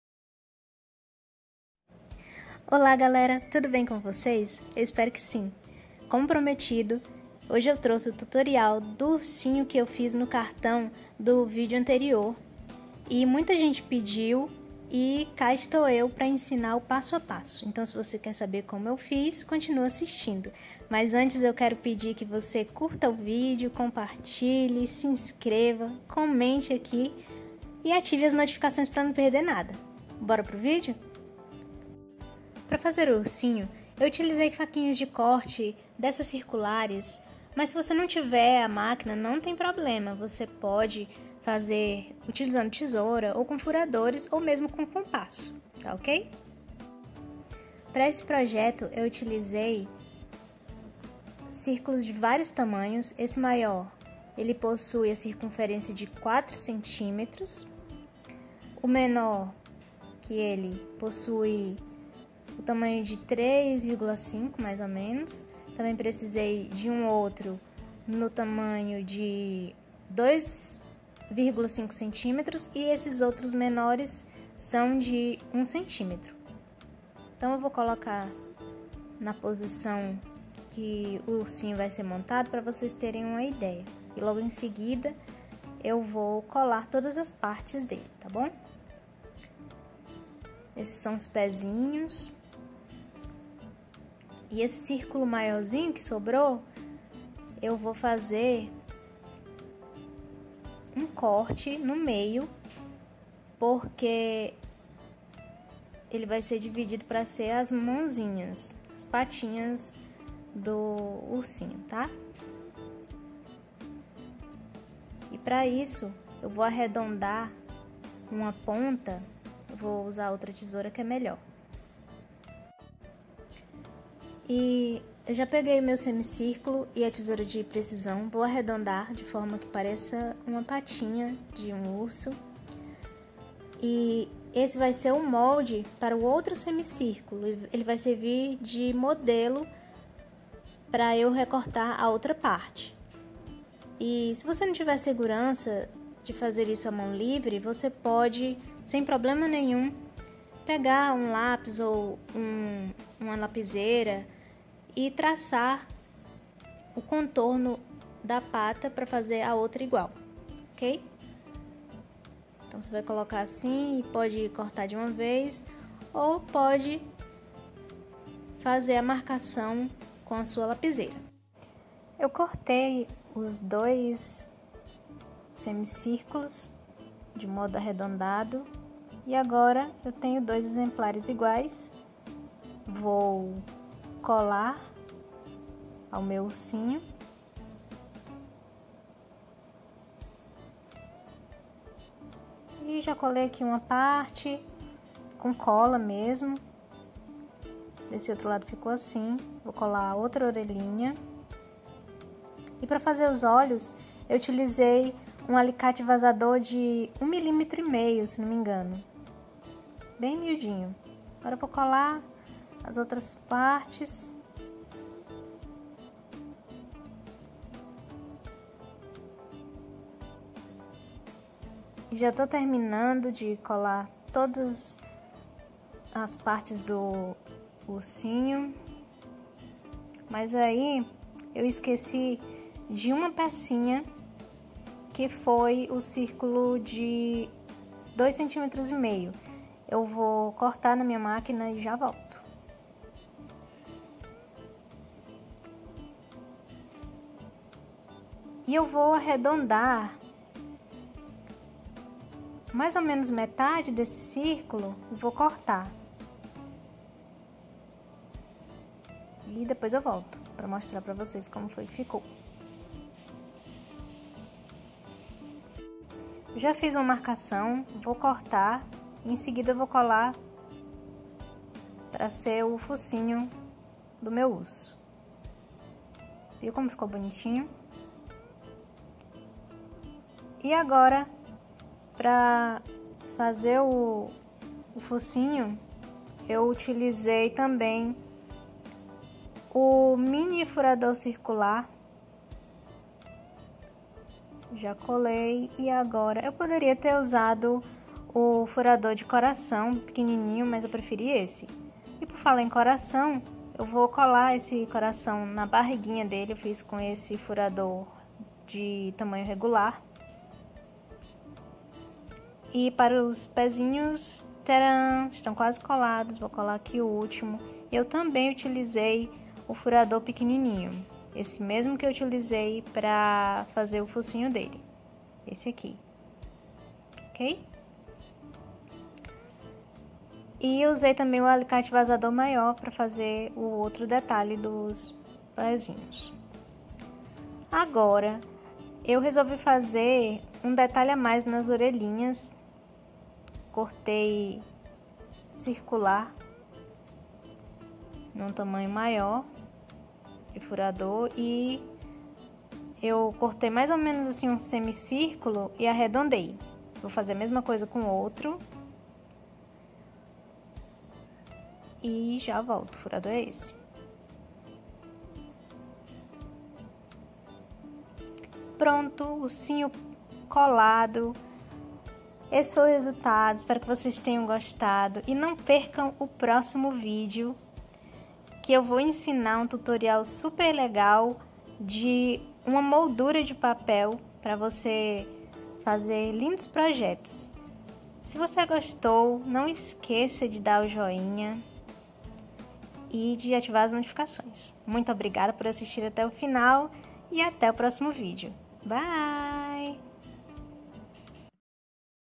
Passo-a-passo ursinho (Little bear tutorial)|Scrapbooking
Olá, galera! Como prometido, hoje trouxe o PAP do urso que mostrei em um dos vídeos anteriores.Fiz um modelo um pouco diferente para mostrar que é possível usar a imaginação e deixar o projeto com uma cara nova. Espero que gostem!